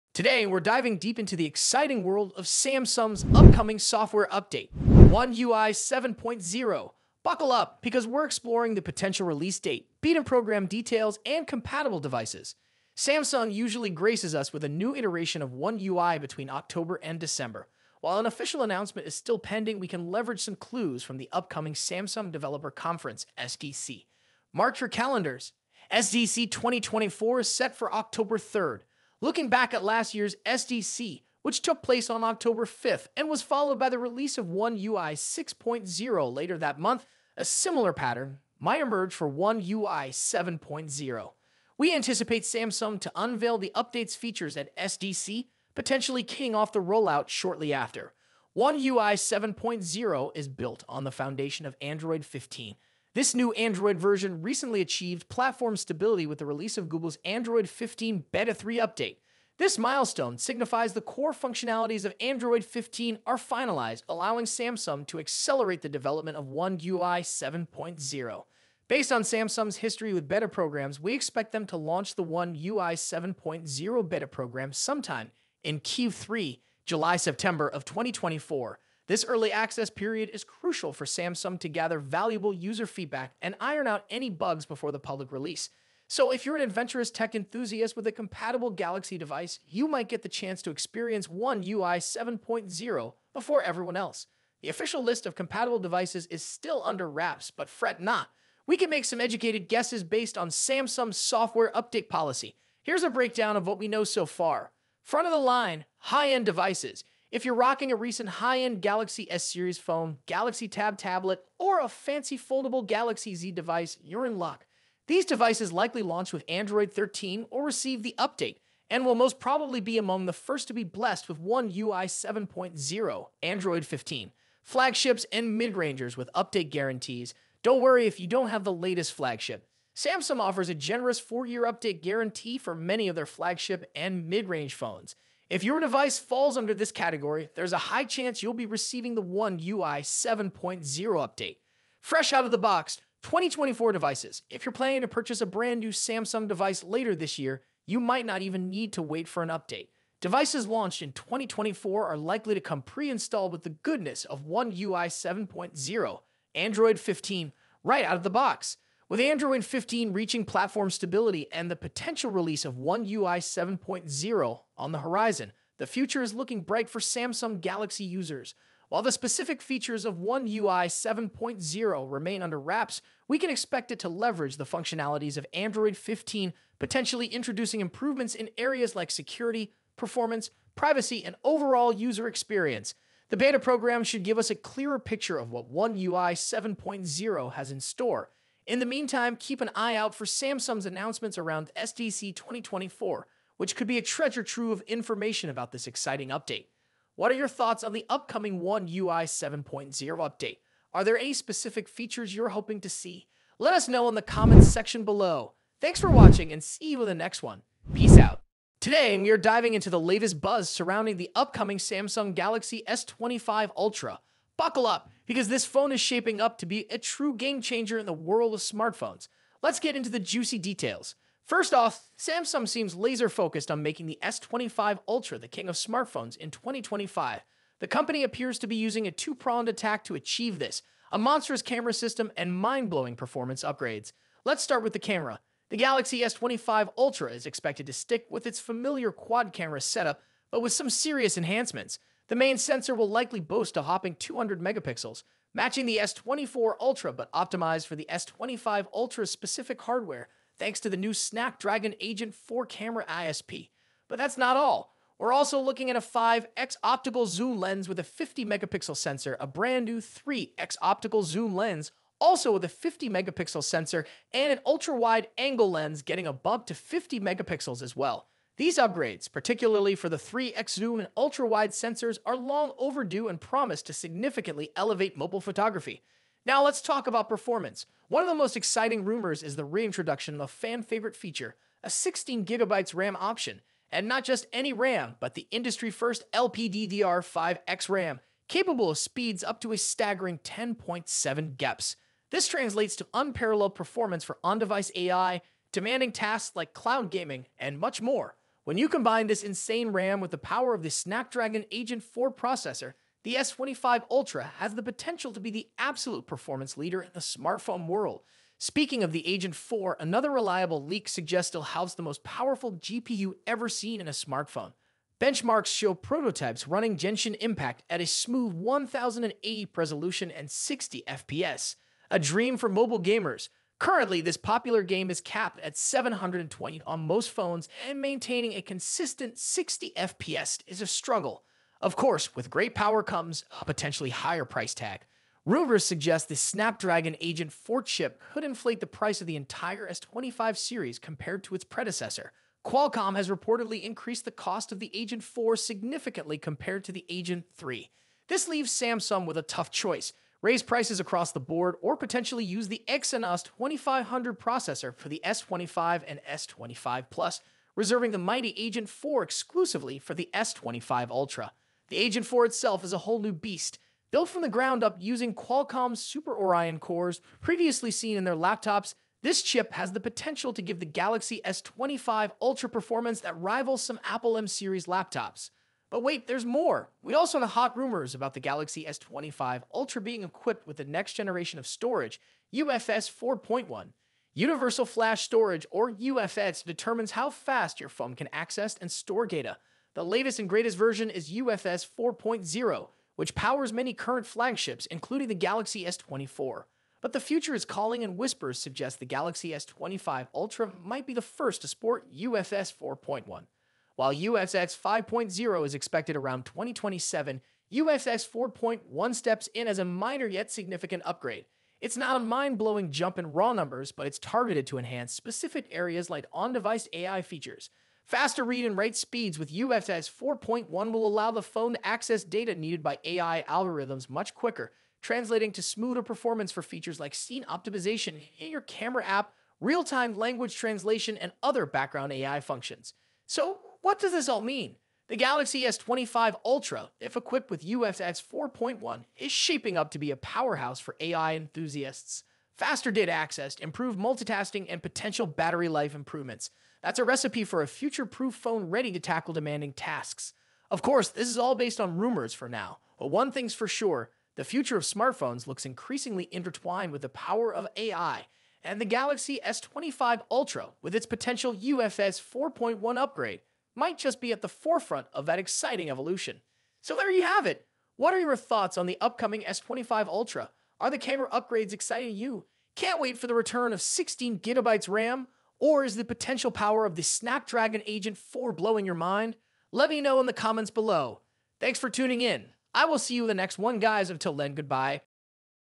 | Samsung's One UI 7.0 Update | Release Date | Beta Program | Compatible Devices |
Today, we're diving into the exciting world of Samsung's upcoming software update - One UI 7.0! Buckle up, because we're exploring the potential release date, beta program details, and compatible devices. Mark your calendars for SDC 2024 on October 3rd, where Samsung might unveil One UI 7.0 built on Android 15.